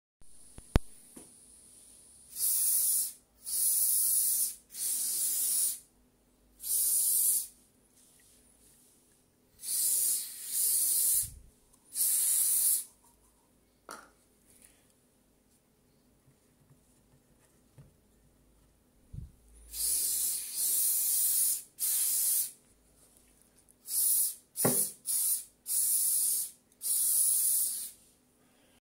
How to create the frosted/icy look on beer photography
Video showing how to apply anti-reflect full matt spray to create the cold effect on beer photography.